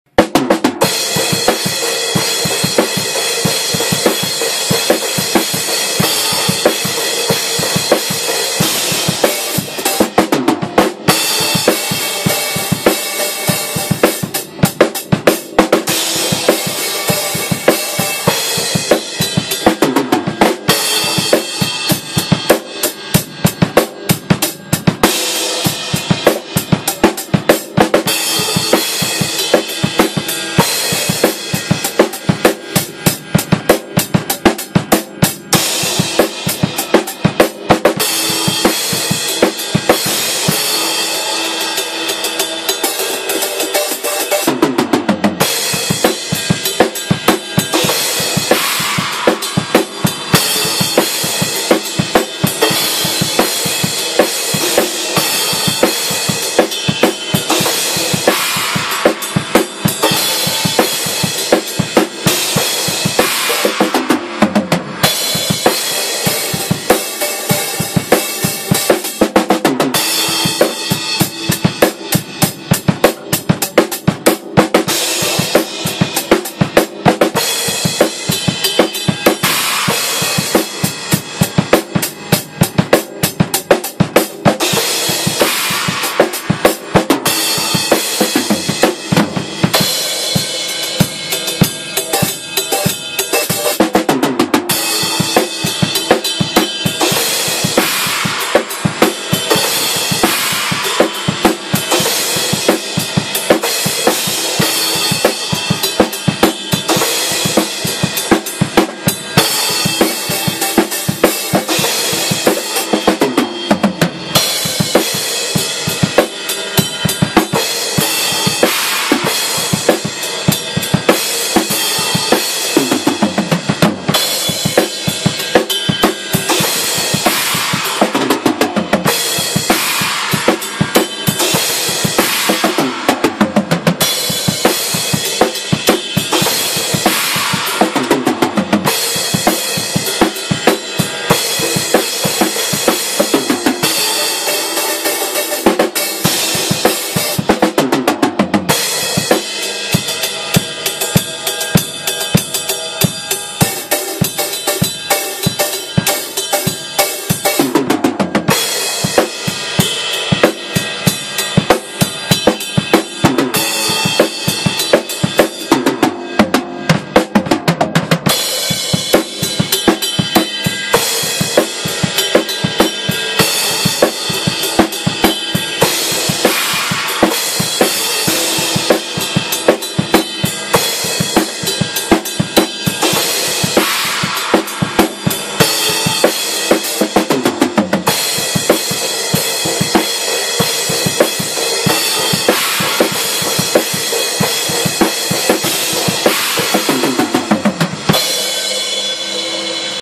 on drums a good take from JAMES DWYER
this is a good take from a song i was listening to in my headphones, I am not sure what song it was i did not make a note of it.